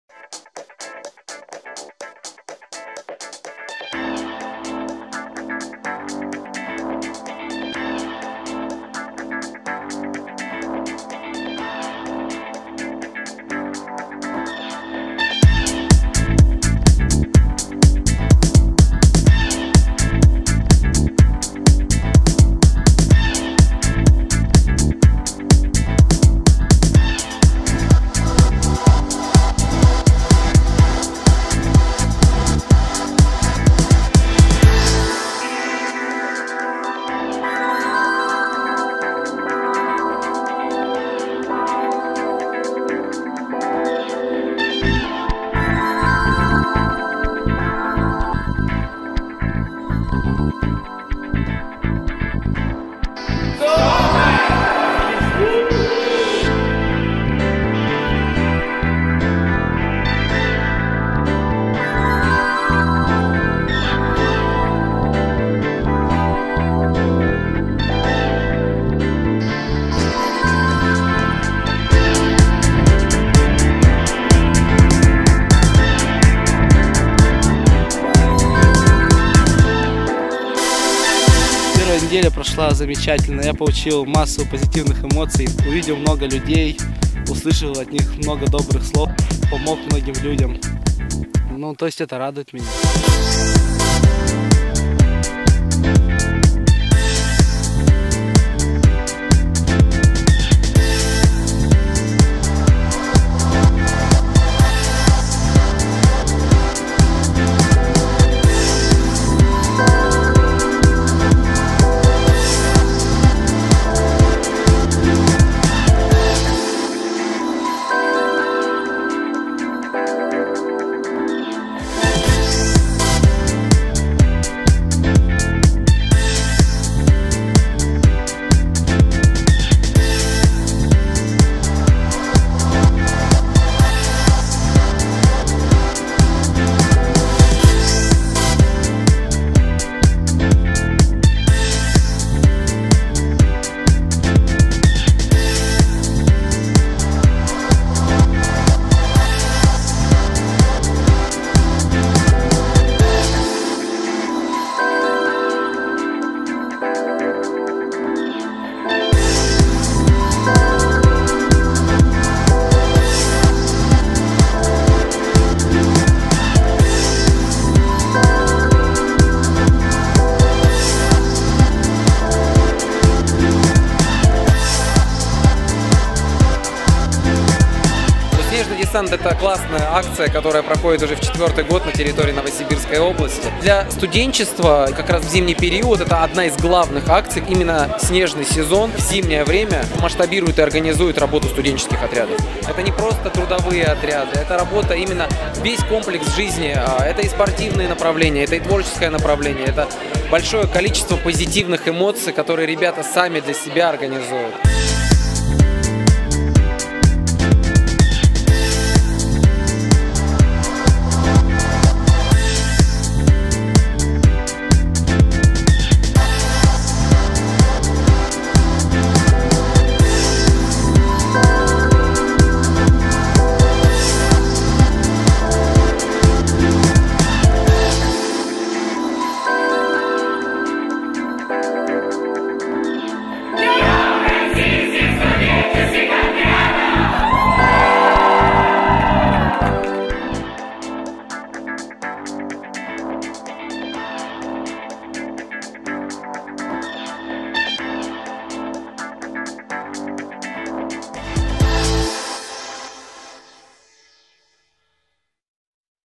"Снежный Десант - Новосибирск"
2 февраля 7 отрядов «Снежного десанта» получили путевки в акцию и отправились в 10 районов Новосибирской области, чтобы провести комплекс социальных мероприятий, таких как: профориентационная работа со старшими школьниками, лекции о здоровом образе жизни, мастер-классы, а так же оказание помощи ветеранам и пожилым людям!